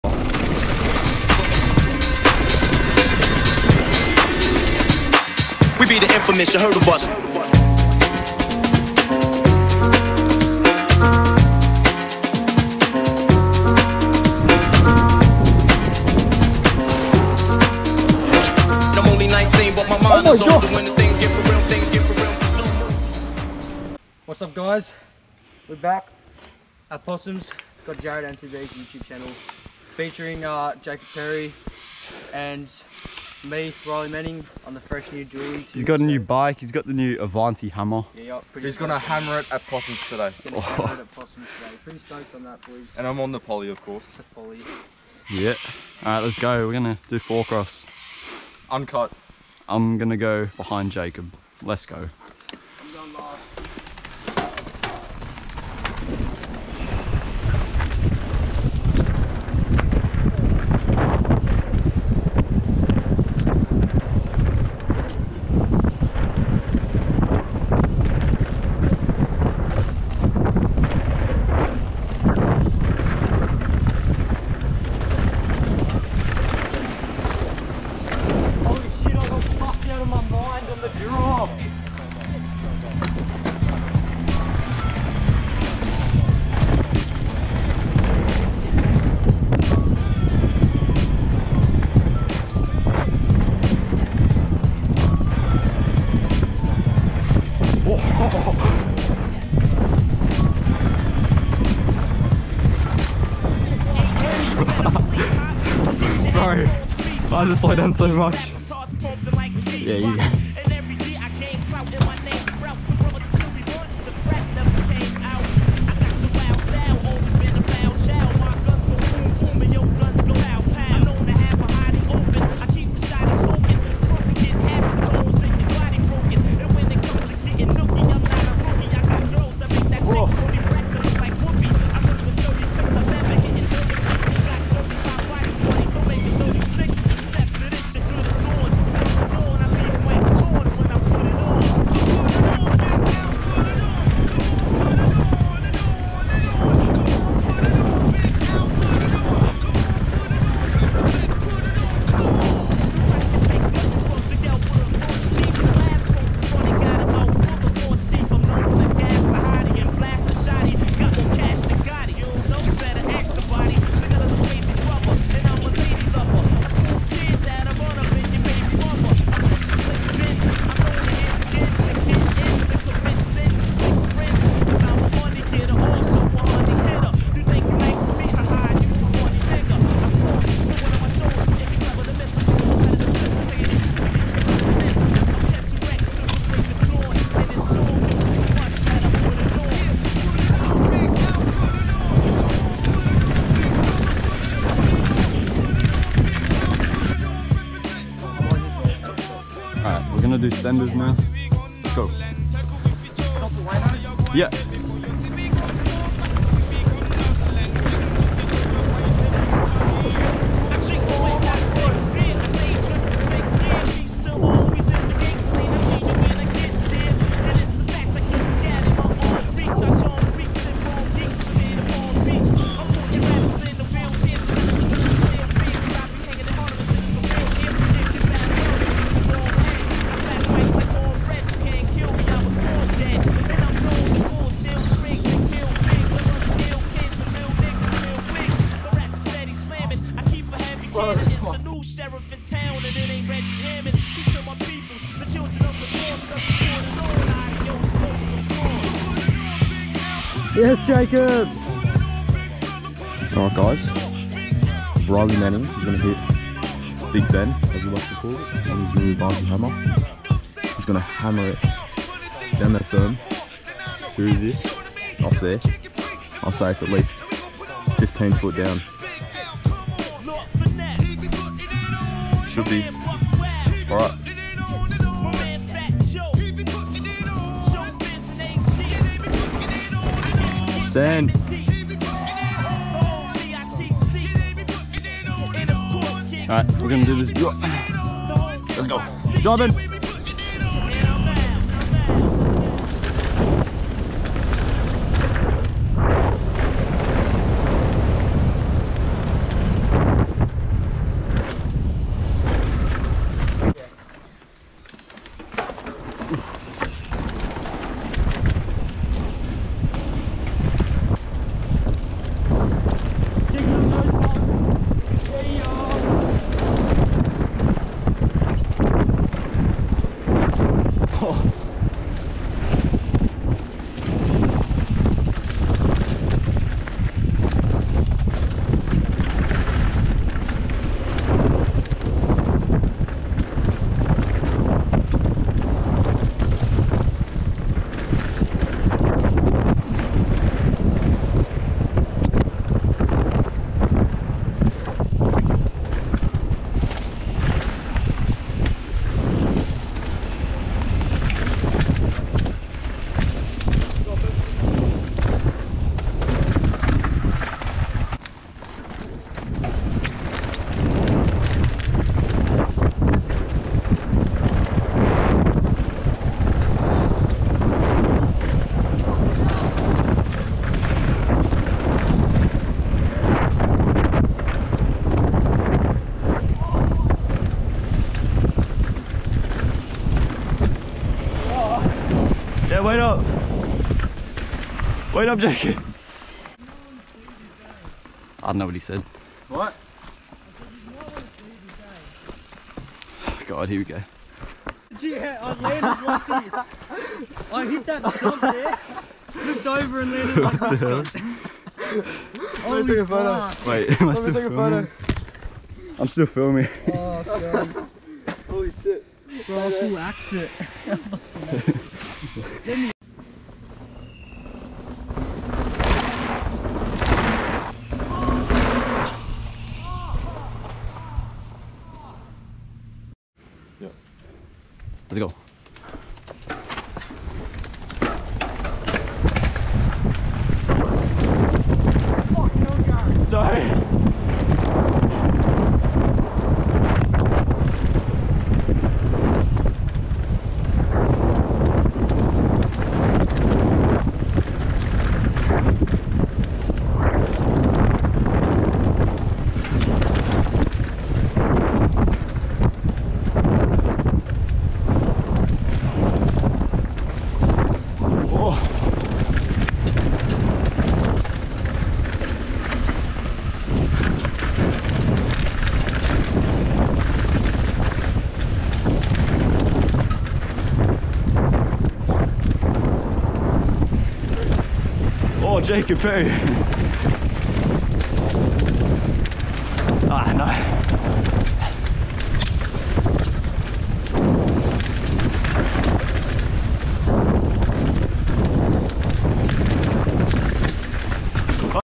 WE WERE AT POSSUMS AT 6:30 (VERY EARLY RIDE)
Riley got a new bike, i broke the rebounder in my forks so i gotta get that replaced and Jacob's polygon is still holding up somehow